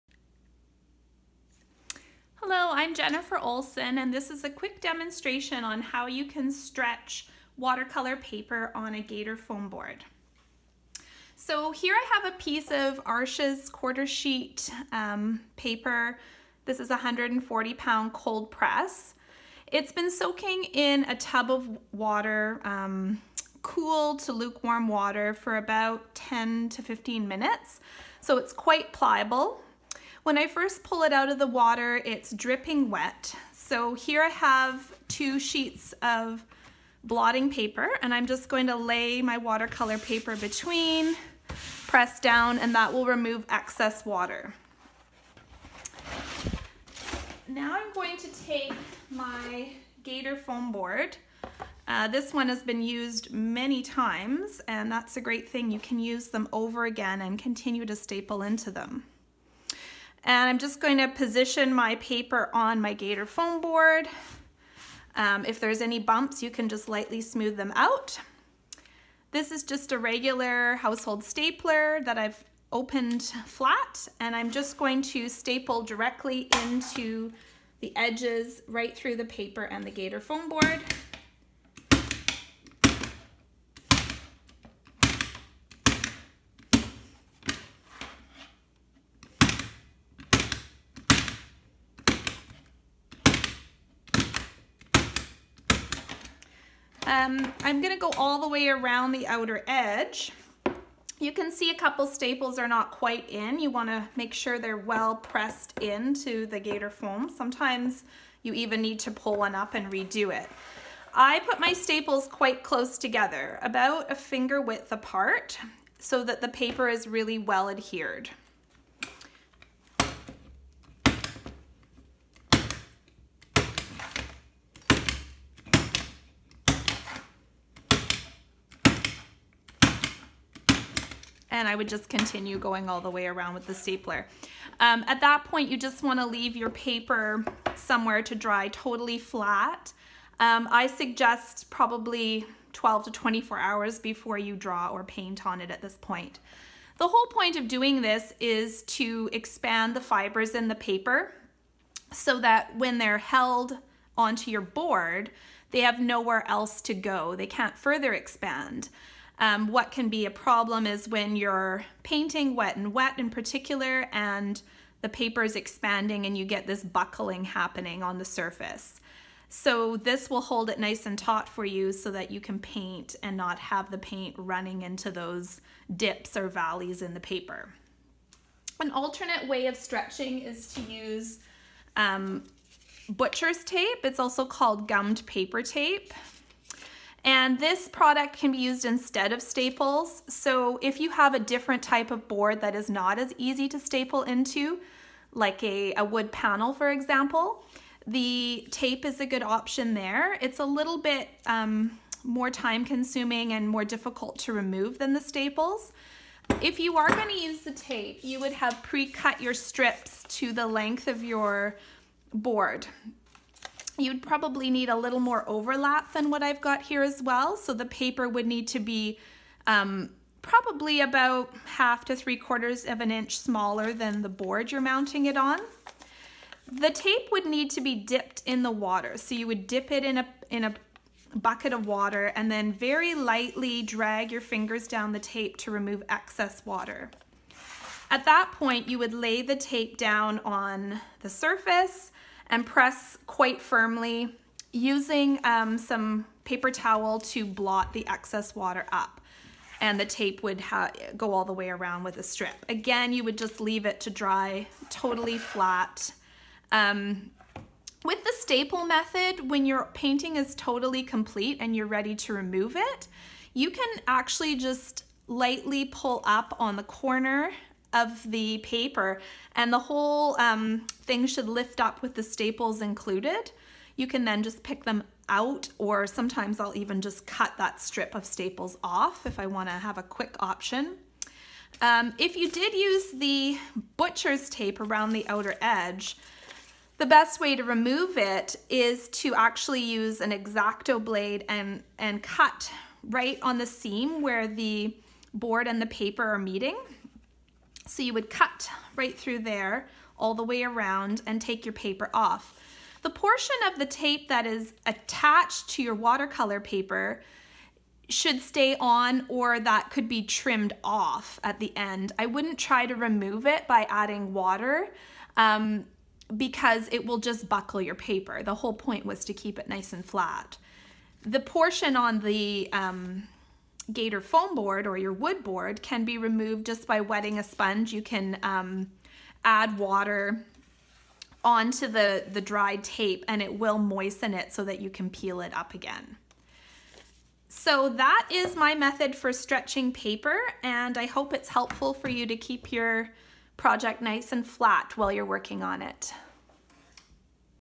How to Stretch Watercolour Paper
In this video Jennifer Olson demonstrates two simple methods for stretching watercolour paper to keep it flat while painting.